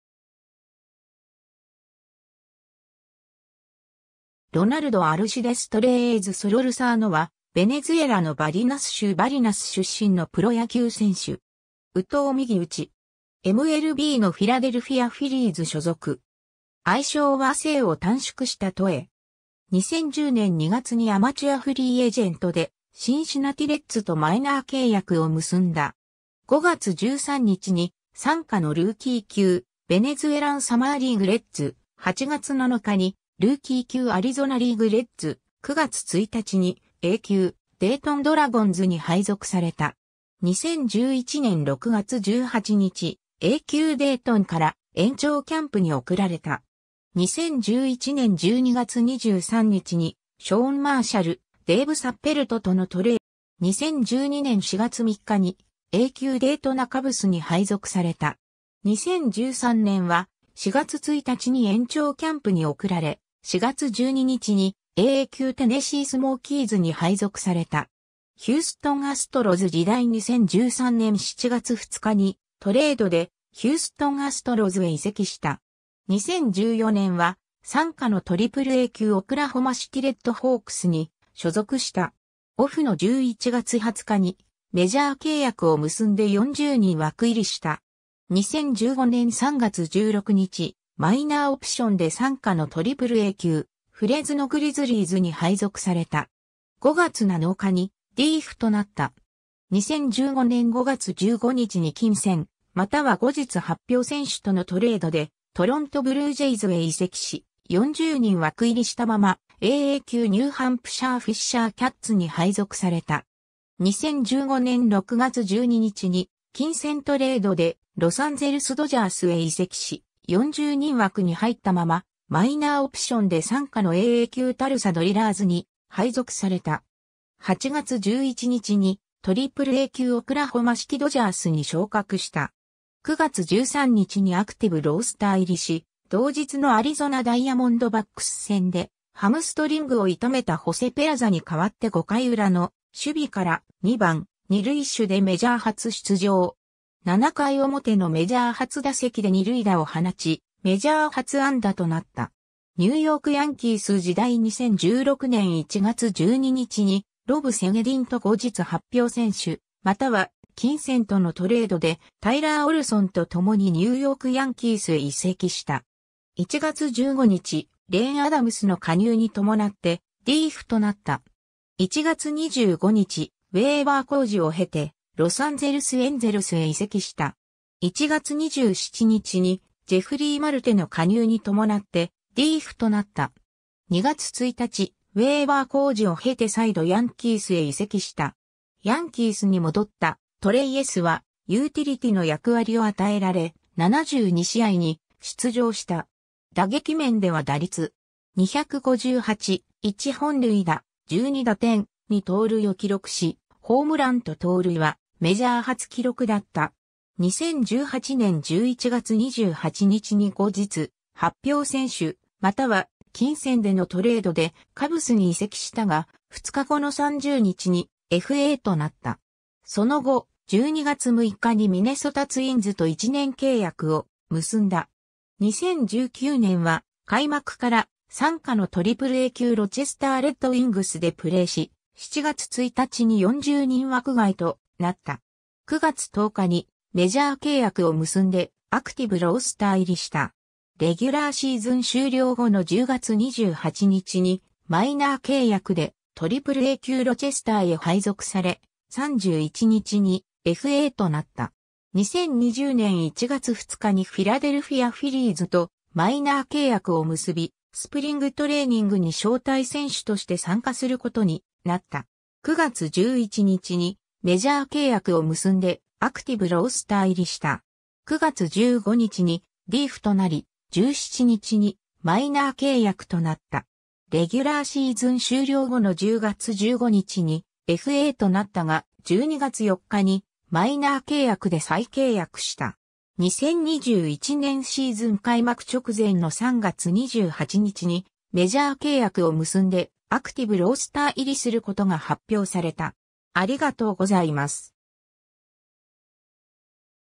ロナルド・トレイエズ
ロナルド・アルシデス・トレイエズ・ソロルサーノ（Ronald Alcides Torreyes Solorsano，1992年9月2日 - ）は、ベネズエラのバリナス州バリナス出身のプロ野球選手（内野手）。
右投右打。
MLBのフィラデルフィア・フィリーズ所属。
愛称は姓を短縮したトエ（Toe）。
 2010年2月にアマチュア・フリーエージェントでシンシナティ・レッズとマイナー契約を結んだ。
5月13日に傘下のルーキー級ベネズエラン・サマーリーグ・レッズ、8月7日にルーキー級アリゾナリーグ・レッズ、9月1日にA級デイトン・ドラゴンズに配属された。
 2011年6月18日、A級デイトンから延長キャンプに送られた。
 2011年12月23日にショーン・マーシャル、デーブ・サッペルトとのトレードで、トラビス・ウッドと共にシカゴ・カブスへ移籍した。
 2012年4月3日にA級デイトナ・カブスに配属された。
 2013年は、4月1日に延長キャンプに送られ、4月12日にAA級テネシー・スモーキーズに配属された。
 ヒューストン・アストロズ時代 (2015年3月7日) 2013年7月2日にトレードで、ヒューストン・アストロズへ移籍した。
 2014年は傘下のAAA級オクラホマシティ・レッドホークスに所属した。
オフの11月20日にメジャー契約を結んで40人枠入りした。
 2015年3月16日、マイナー・オプションで傘下のAAA級フレズノ・グリズリーズに配属された。
5月7日にDFAとなった。
 2015年5月15日に金銭または後日発表選手とのトレードで、トロント・ブルージェイズへ移籍し、40人枠入りしたままAA級ニューハンプシャー・フィッシャーキャッツに配属された。
 2015年6月12日に金銭トレードで、ロサンゼルス・ドジャースへ移籍し、40人枠に入ったままマイナー・オプションで傘下のAA級タルサ・ドリラーズに配属された。
8月11日にAAA級オクラホマシティ・ドジャースに昇格した。
9月13日にアクティブ・ロースター入りし、同日のアリゾナ・ダイヤモンドバックス戦でハムストリングを痛めたホセ・ペラザに代わって5回裏の守備から「2番・二塁手」でメジャー初出場。
7回表のメジャー初打席で二塁打を放ちメジャー初安打となった。
 ニューヨーク・ヤンキース時代 (2018年8月25日) 2016年1月12日にロブ・セゲディンと後日発表選手または金銭とのトレードで、タイラー・オルソンと共にニューヨーク・ヤンキースへ移籍した。
1月15日、レーン・アダムスの加入に伴ってDFAとなった。
1月25日、ウェイバー公示を経てロサンゼルス・エンゼルスへ移籍した。
1月27日にジェフリー・マルテの加入に伴ってDFAとなった。
2月1日、ウェイバー公示を経て再度ヤンキースへ移籍した。
ヤンキースに戻ったトレイエスはユーティリティの役割を与えられ、72試合に出場した。
打撃面では打率.258、1本塁打、12打点、2盗塁を記録し、ホームランと盗塁はメジャー初記録だった。
 2018年11月28日に後日発表選手または金銭でのトレードで、カブスに移籍したが、2日後の30日にFAとなった。
 その後、12月6日にミネソタ・ツインズと1年契約を結んだ。
 2019年は開幕から傘下のAAA級ロチェスター・レッドウイングスでプレーし、7月1日に40人枠外となった。
9月10日にメジャー契約を結んでアクティブ・ロースター入りした。
レギュラーシーズン終了後の10月28日にマイナー契約でAAA級ロチェスターへ配属され、31日にFAとなった。
 2020年1月2日にフィラデルフィア・フィリーズとマイナー契約を結び、スプリングトレーニングに招待選手として参加することになった。
9月11日にメジャー契約を結んでアクティブ・ロースター入りした。
9月15日にDFAとなり、17日にマイナー契約となった。
レギュラーシーズン終了後の10月15日にFAとなったが、12月4日にマイナー契約で再契約した。
 2021年シーズン開幕直前の3月28日にメジャー契約を結んでアクティブ・ロースター入りすることが発表された。